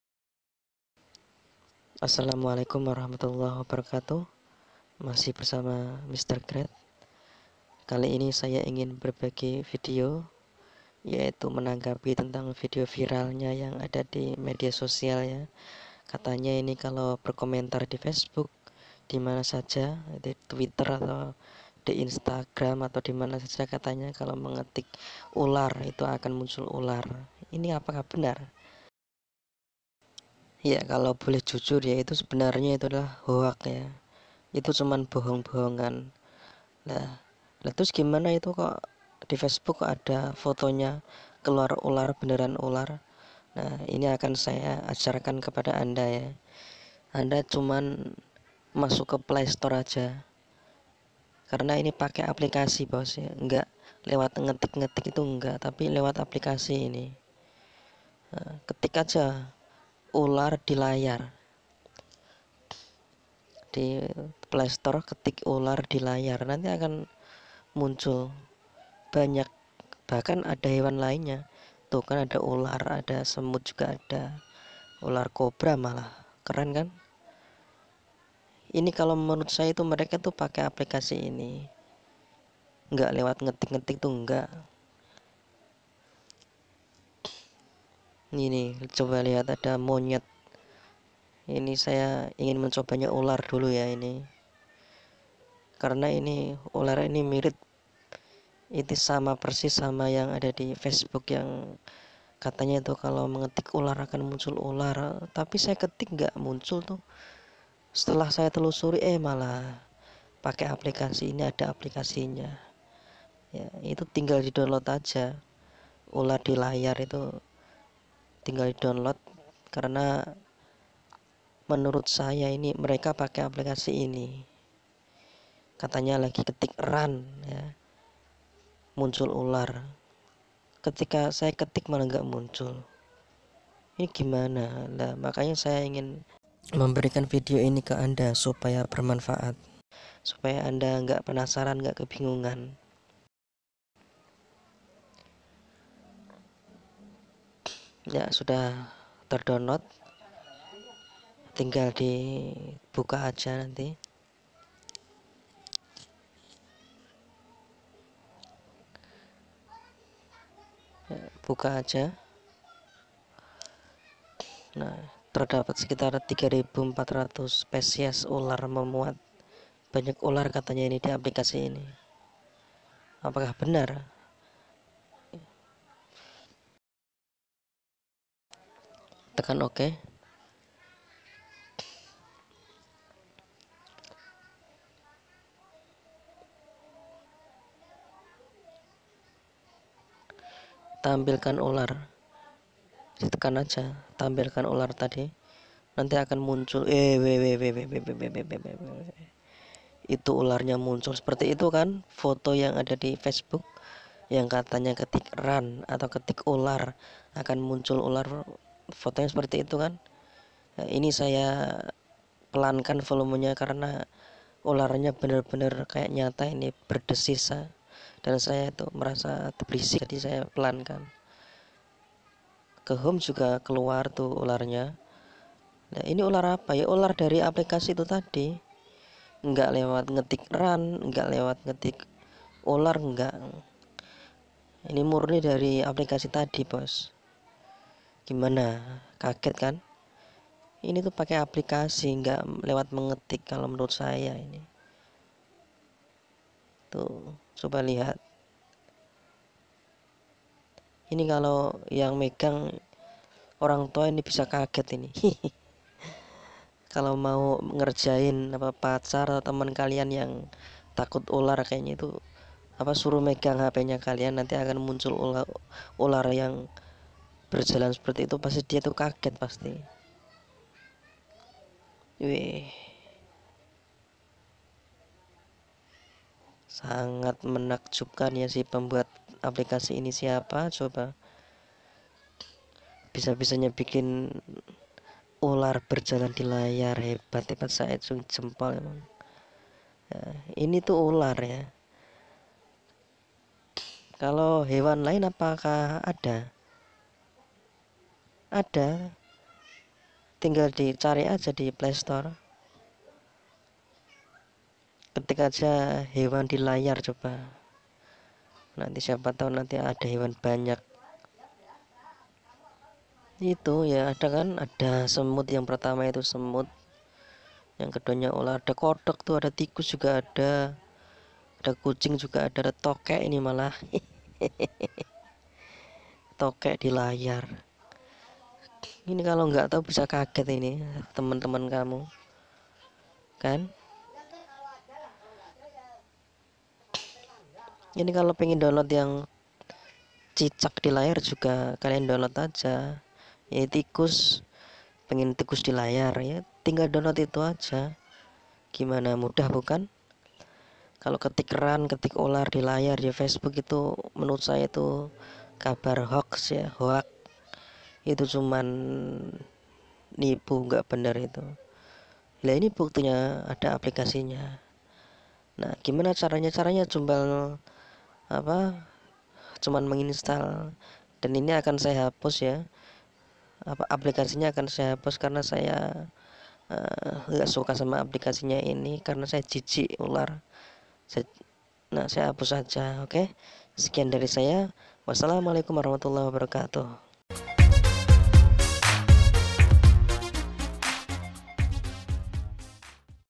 Ular Di Layar | Ketik Ular Muncul Ular | Ketik Ran Muncul Ular | Viral Ketik Ular Di Media Sosial
Apa kalian pernah di suruh mengeti ular katanya akan muncul ular, di facebook di twitter di instagram katanya juga dalam video ini akan menjawab semua penasaran kamu jadi pastikan tonton videonya sampai selesai ya supaya tau caranya bikin ular bisa di layar android